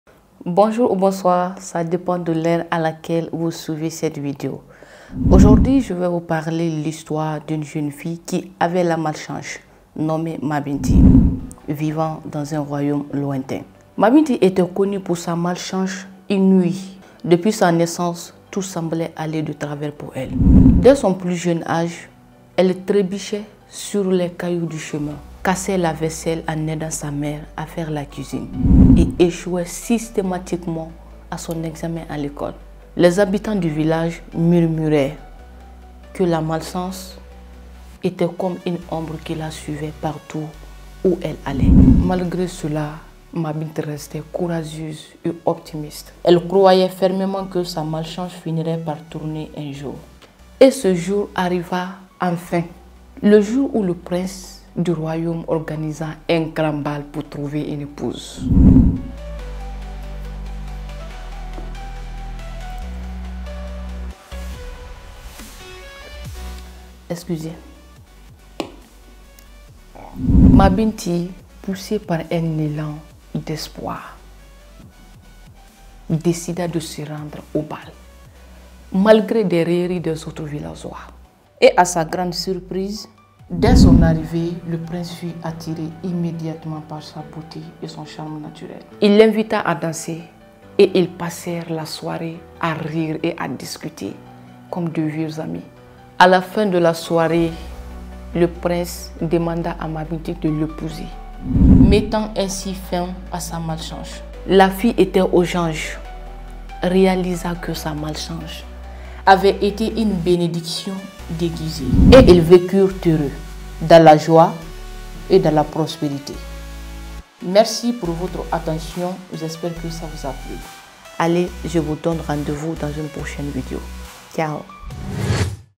Malchanceuse depuis mon enfance, j'ai enfin trouvé l'amour royal😍🎉
Mabinty, une jeune fille réputée pour sa malchance, vivait dans un royaume lointain. Depuis son enfance, elle enchaînait les mésaventures, et les villageois la considéraient comme un symbole de malchance. Pourtant, malgré les difficultés, Mabinty resta optimiste et espéra un jour meilleur. Son destin changea lorsqu'un bal fut organisé par le prince du royaume pour choisir une épouse. Contre toute attente, le prince fut immédiatement séduit par Mabinty, et ils passèrent la soirée ensemble. Le prince lui proposa de l'épouser, transformant ainsi sa malchance en une bénédiction. Ils vécurent heureux et prospères.